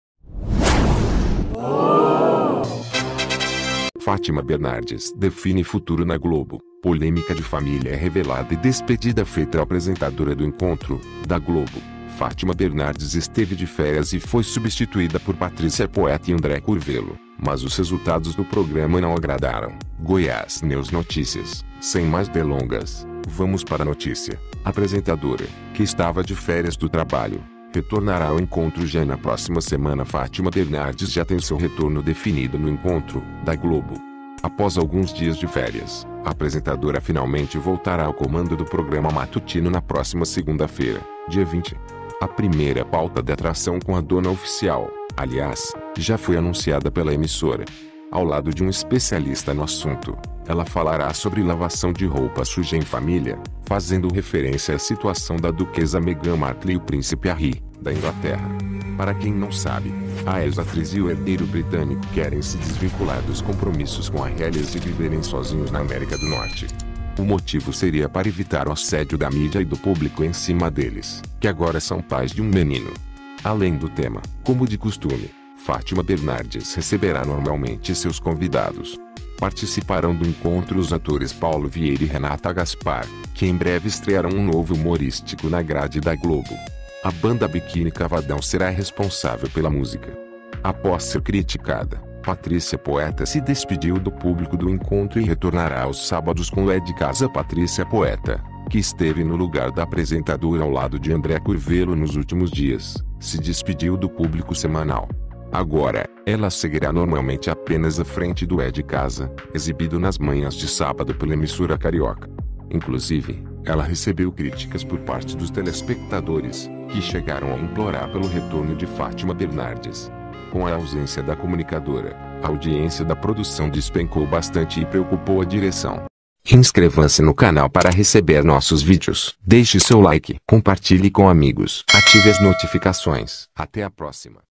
Fátima Bernardes define futuro na Globo, polêmica de família é revelada e despedida é feita
👀1⃣2⃣3⃣👀▁▂▃▅▆██████▆▅▃▂▁

           _*🤳🏼A apresentadora do Encontro, da Globo, Fátima Bernardes esteve de férias e foi substituída por Patrícia Poeta e André Curvello, mas os resultados do programa não agradaram*_

*🥏MEU SEGUNDO CANAL GOIÁS NÉWS POLÍTICO* ↙️

*💻CLIQUE NO LINK LOGO ABAIXO PARA SEGUIR NO INSTAGRAM ⬇*